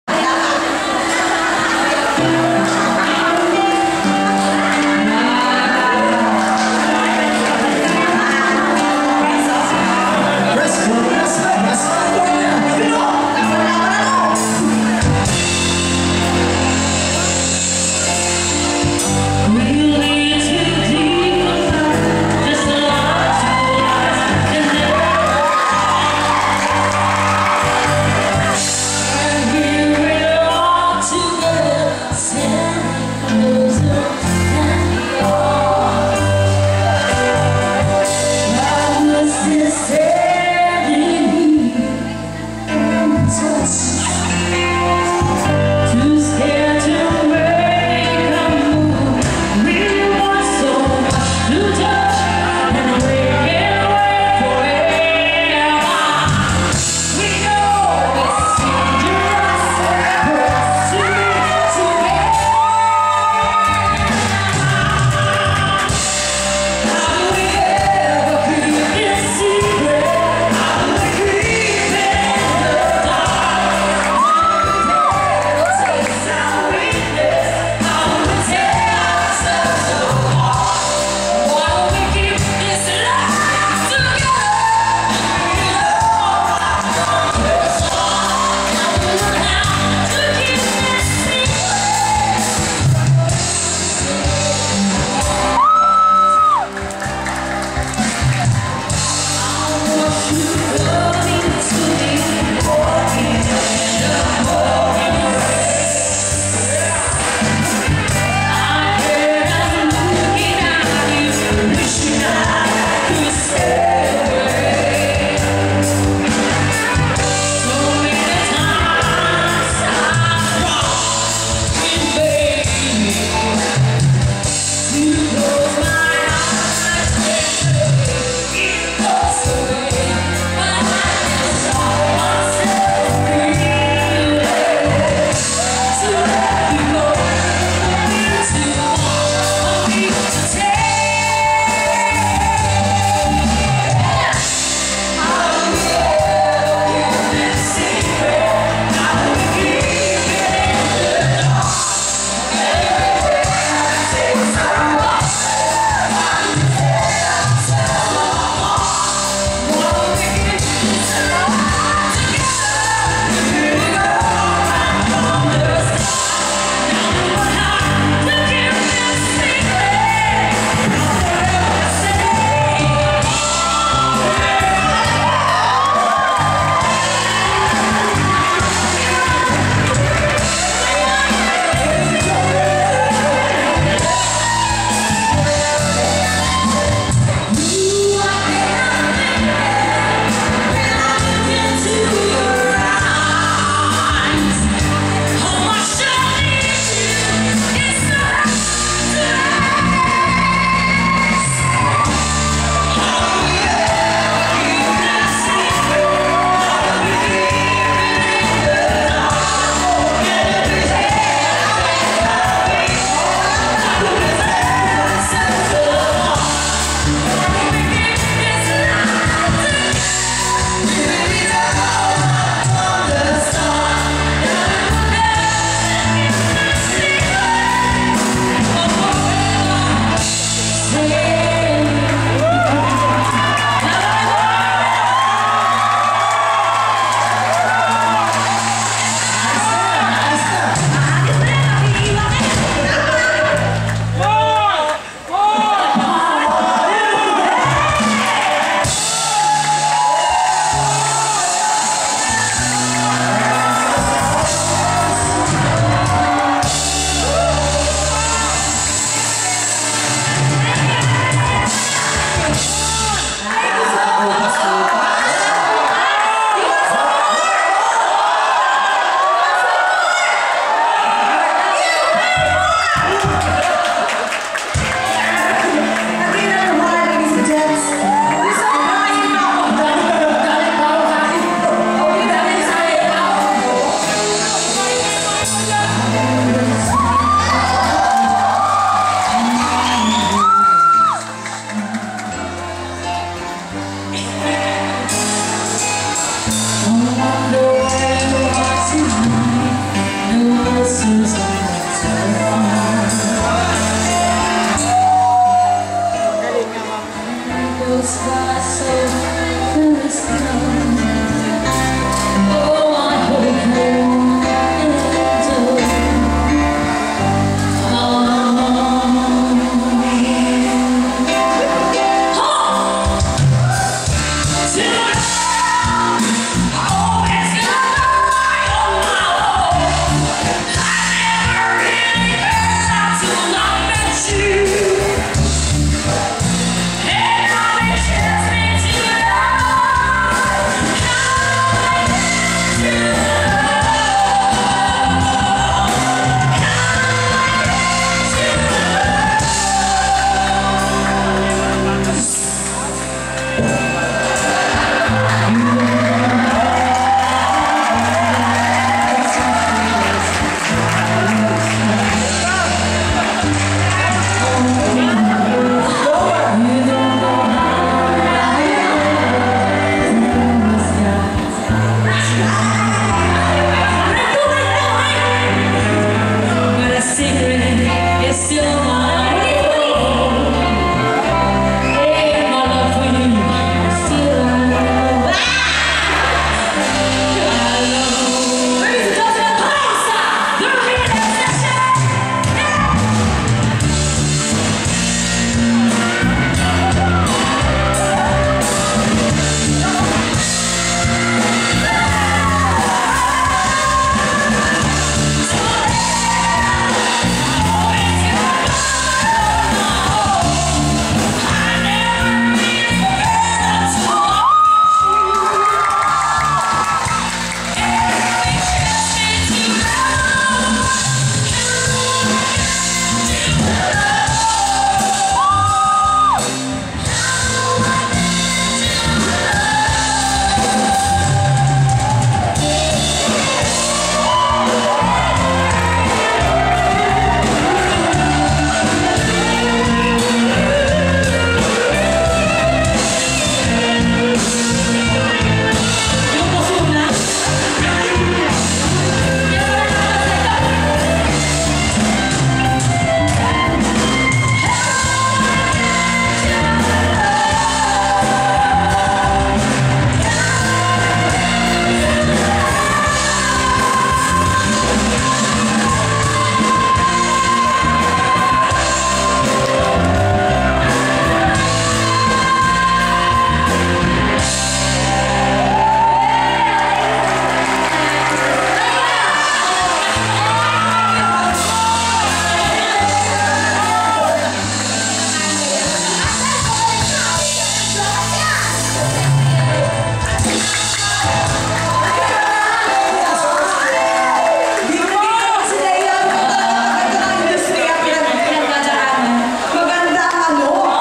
lhea2013 gapo
I USED TO BE A SINGER,,,I LOVED DOING IT,,BUT IM ON A DIFFERENT WORLD NOW,,SO GOING BACK AND SEEING MY OLD TIME FRIENDS MADE ME HAPPY,,AND SINGING WITH THEM,,YOU GUYS ARE GREAT,,TO SONNY BOY AND BAND MEMBERS THANKS ALOT..U MADE MY NYT...